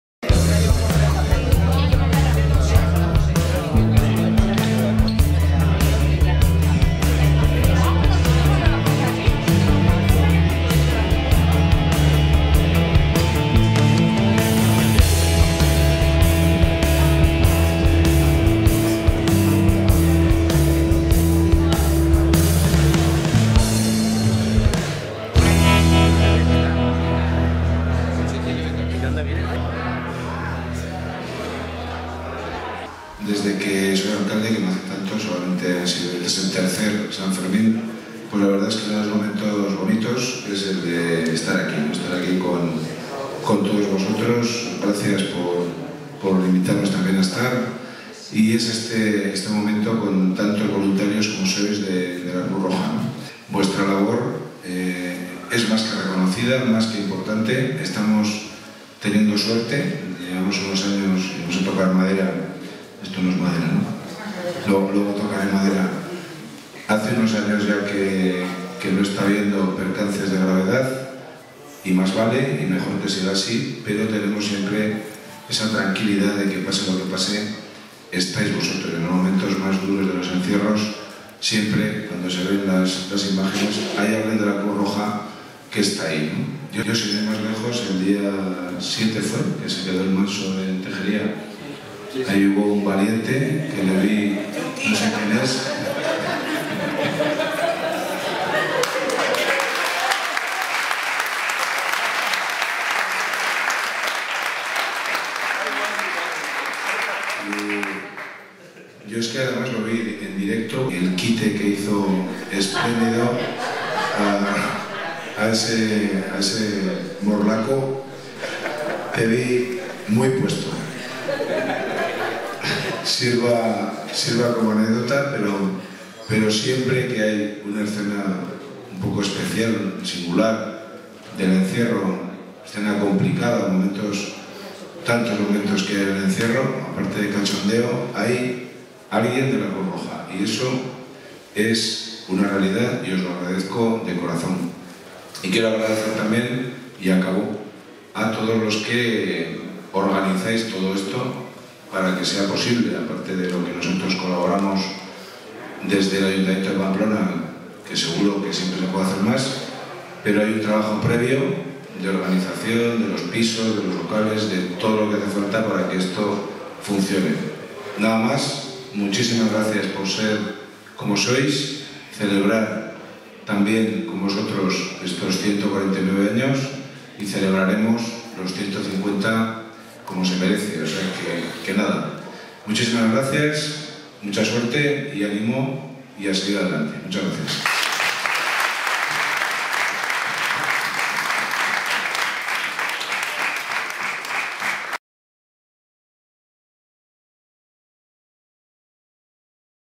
El alcalde de Pamplona Enrique Maya, visita el dispositivo de Cruz Roja en el encierro
+El alcalde Enrique Maya, junto con Ignacio Polo y Alberto Margallo visitan hoy el dispositivo del encierro, agradece el trabajo del voluntariado y bromea con nuestro compañero Dani Vicente que tuvo que recortar al manso escapado en el encierro del día 8 de julio